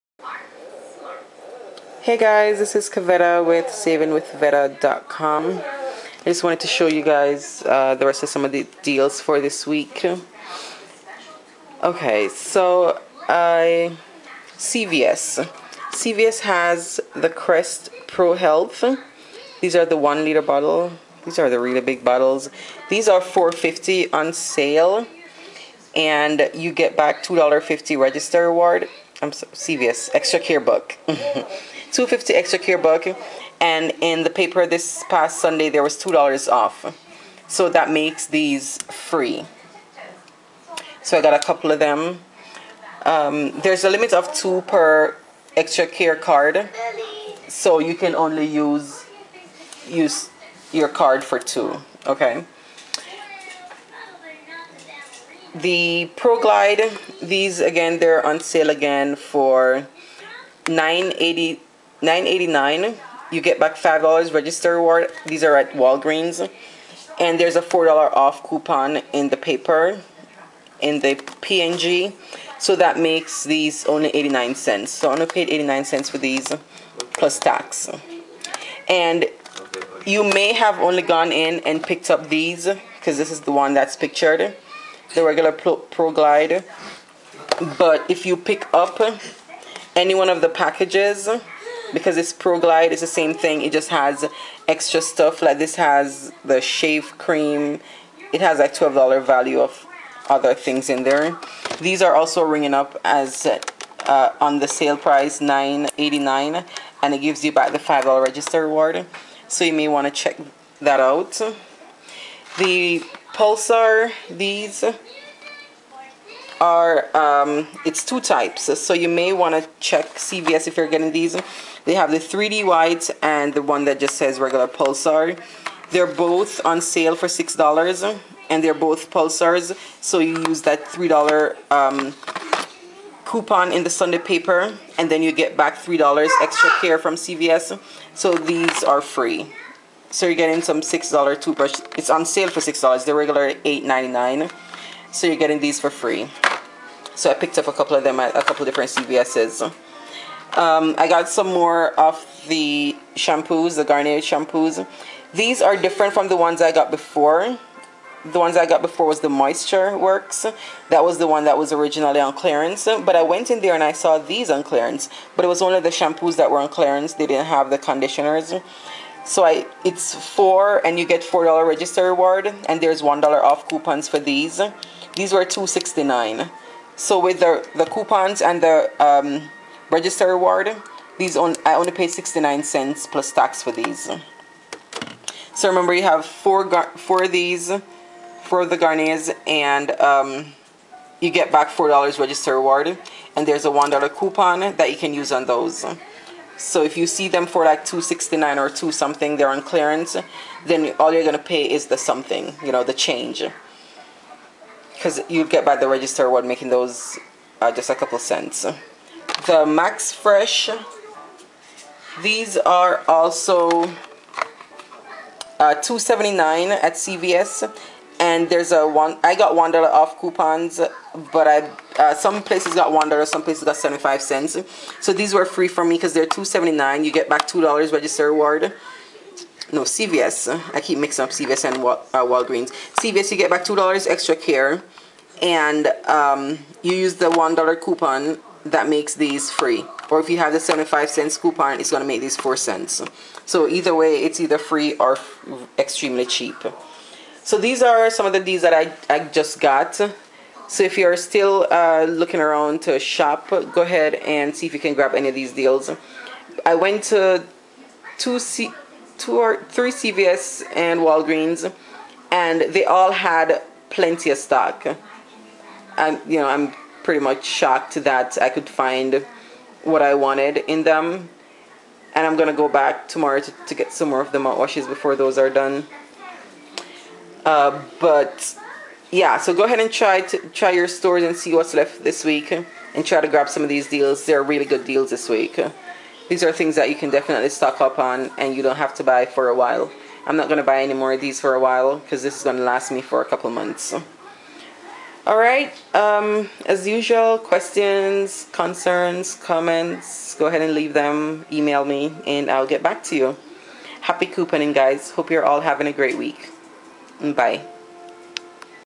Walgreens and CVS 6-9
Deals and steals this week. I basically came out with everything free at Wags and $30RR. I had a gift card from them that was free so I used that to pay for evrything and got back money. So I really made money shopping at Wags tonight. Not even tax came out of my pocket. CVS I did pay tax on the items and everything was free after coupons and ECB.